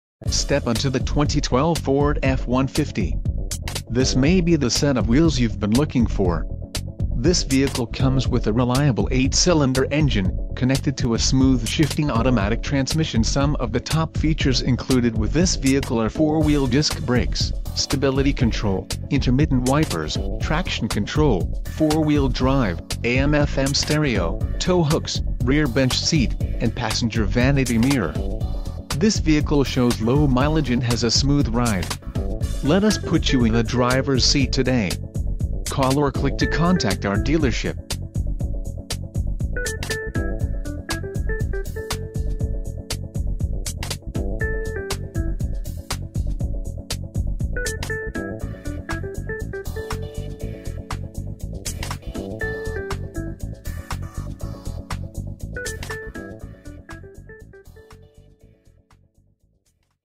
2012 Ford F-150 - Friendly Ford - Geneva, NY 14456
ONE OWNER 2012 Ford F150 XL Longbed with Towing Package, Running Boards, Sirius Satellite Radio, Bed Liner, and Leather Seats!!